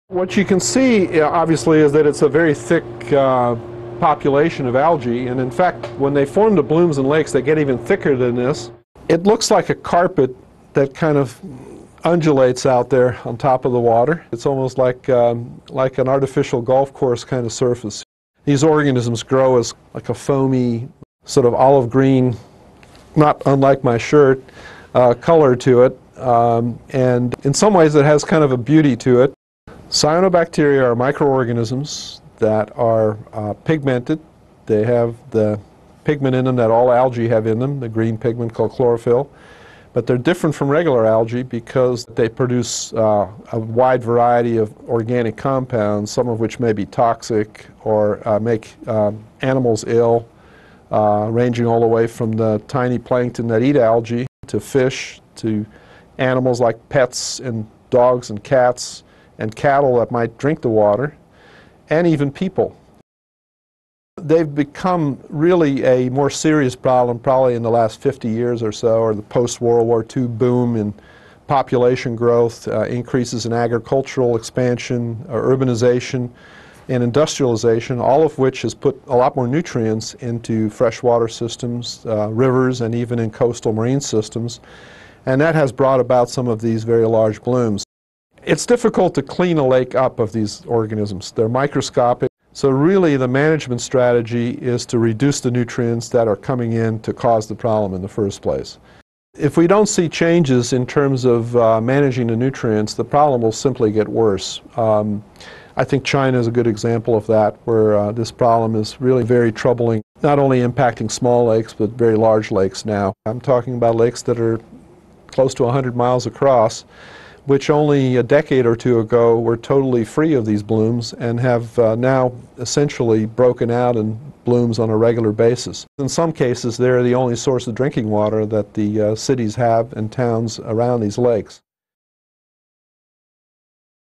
Cyanobacteria
Hans Paerl, a University of North Carolina at Chapel Hill Institute of Marine Sciences Professor